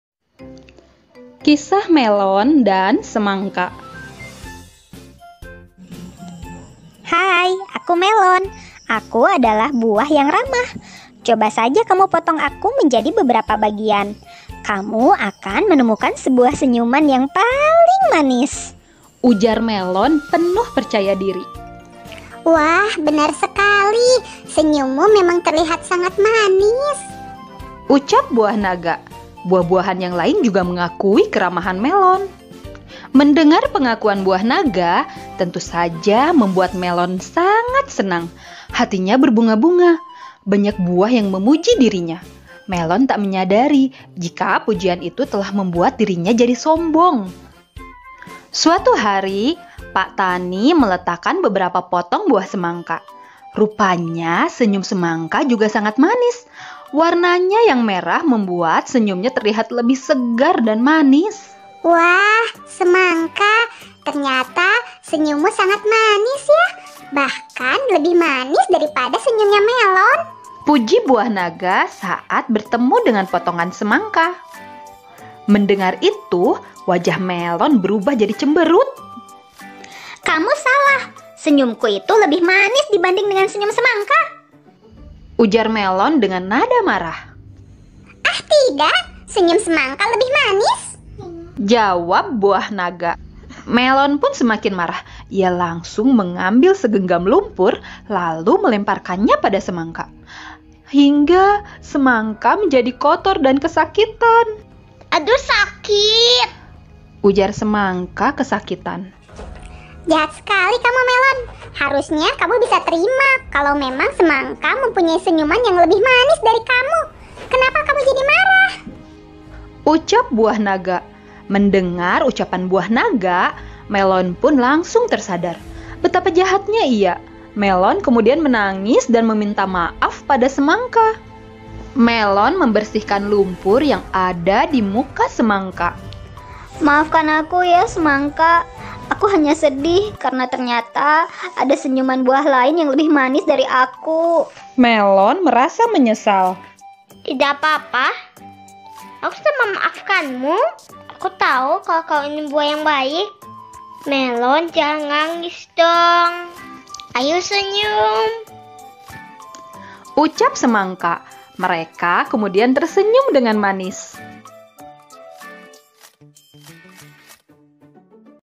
Kisah Melon dan Semangka l Dongeng Sebelum Tidur
Melon merasa kesal pada Semangka hingga melempar lumpur ke muka temannya itu. Apa yang sudah dilakukan oleh Semangka sehingga membuat Melon marah?